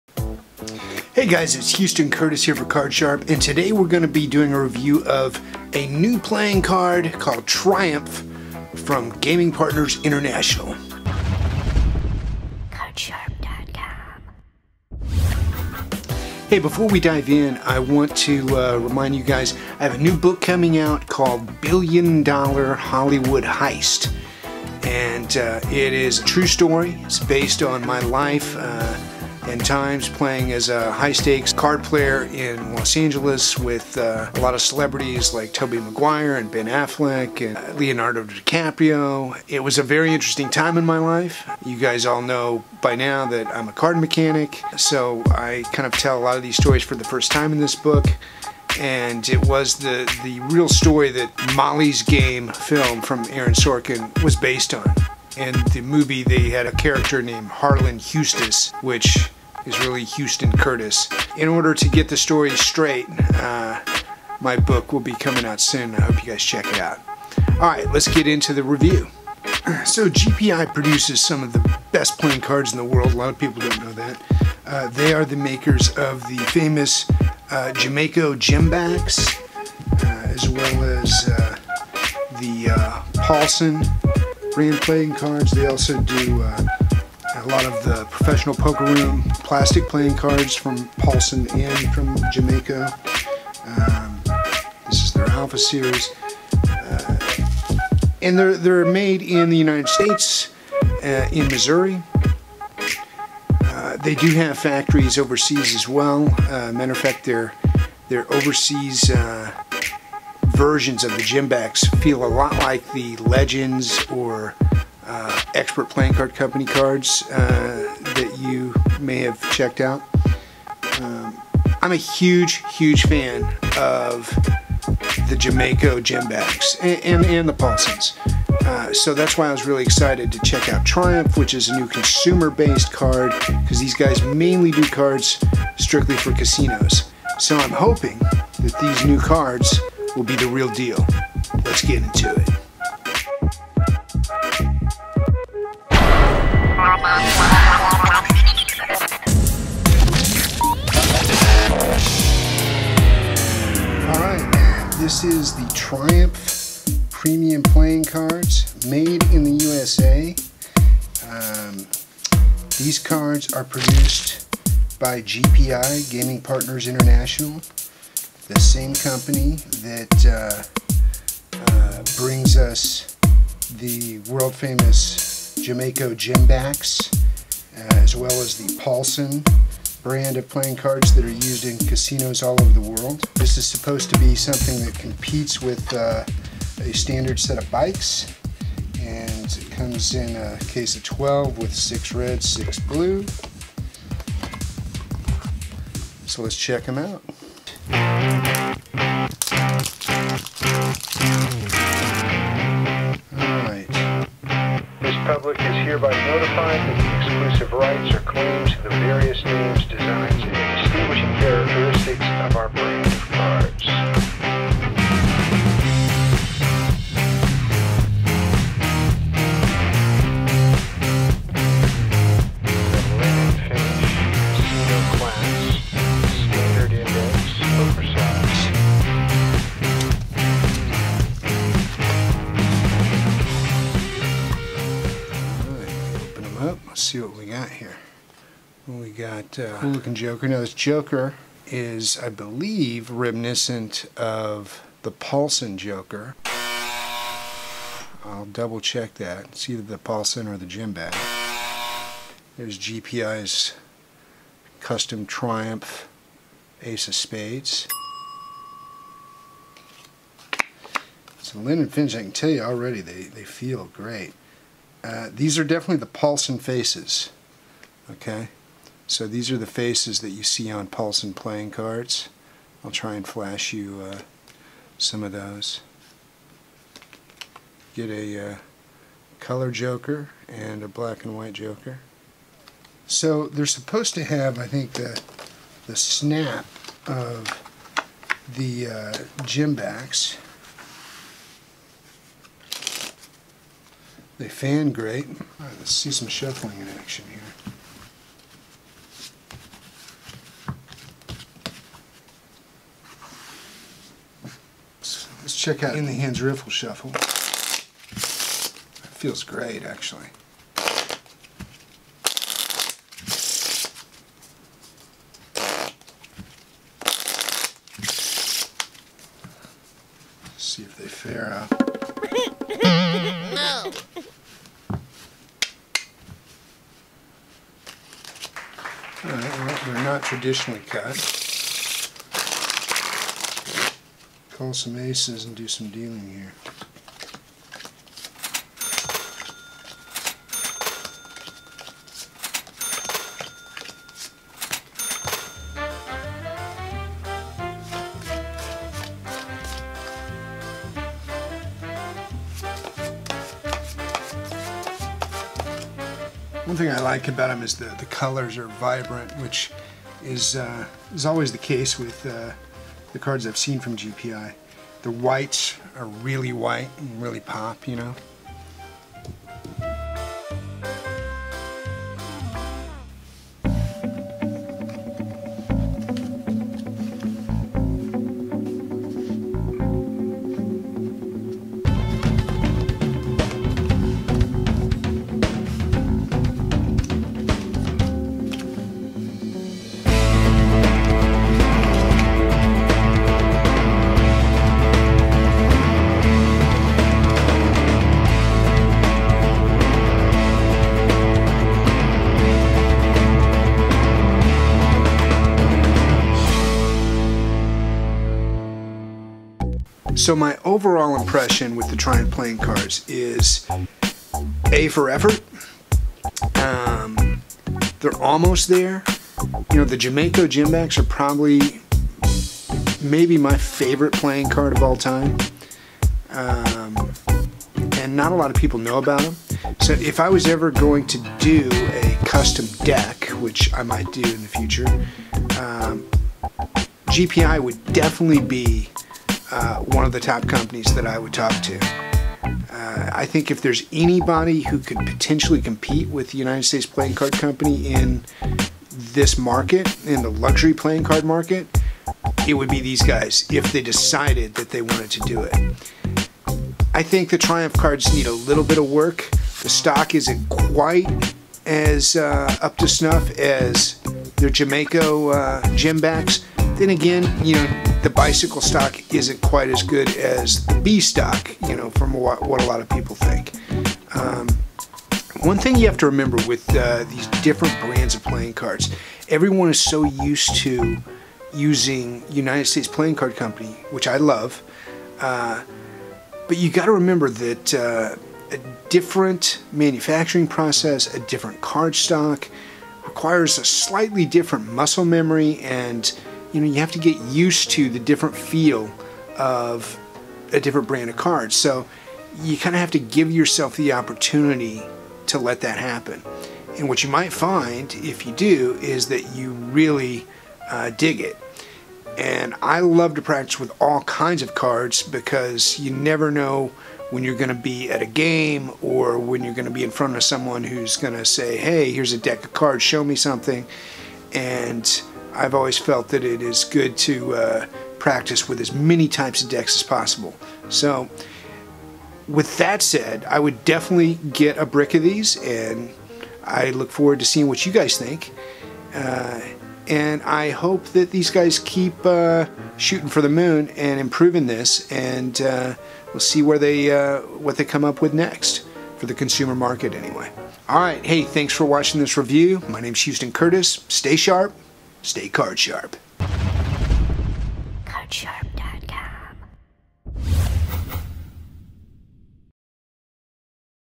Triumph Deck Review - Better than Bicycle Cards?
KardSharp's latest deck review takes a look at the new line of consumer based playing cards from Casino manufacturer , GPI (Gaming Partners International).  GPI is known primarily for their casino playing cards which are featured in casinos all over the world including Gemaco Gembacks and Paulson Brand cards.  Triumph is their new answer to USPCC Bicycle Rider Backs.  Triumph, Gemaco, Paulson, USPCC and other luxury, vintage and specially curated playing cards are available soon